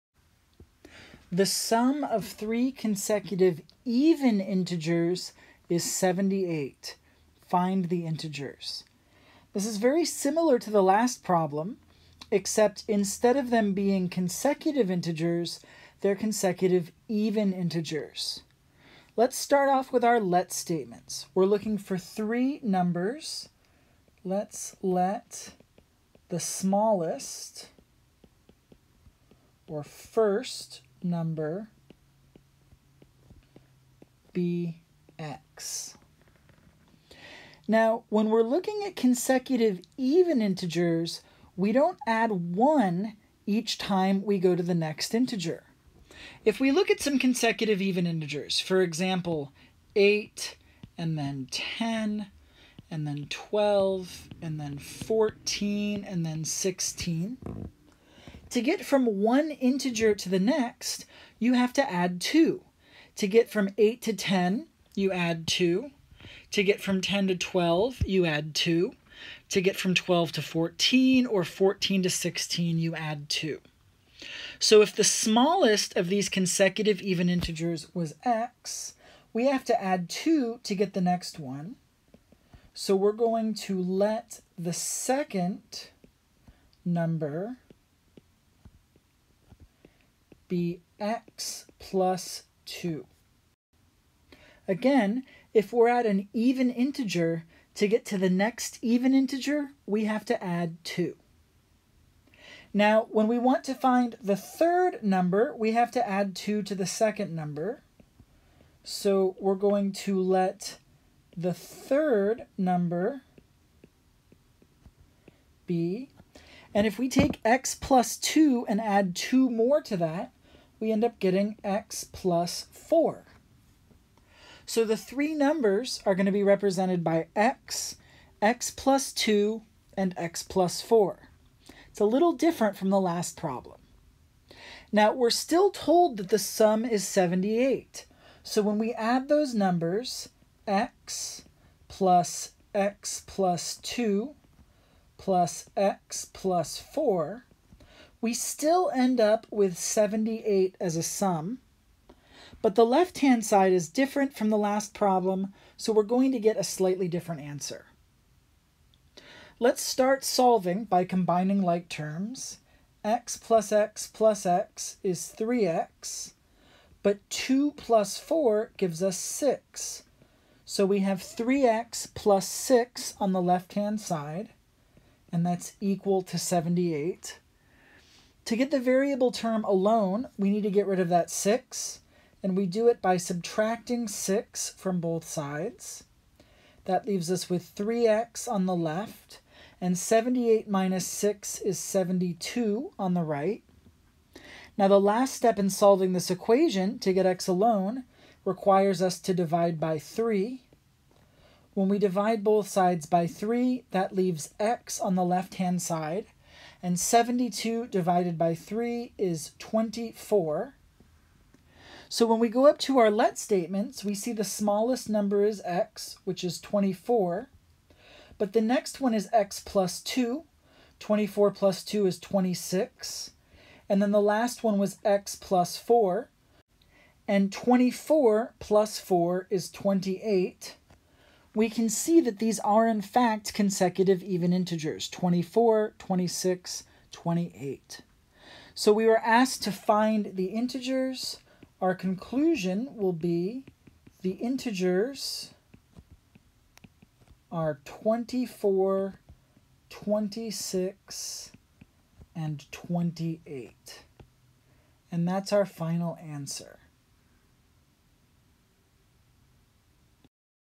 Consecutive Integer Problems - Problem 2
Watch how to solve this word problem on the topic of problems involving consecutive integers.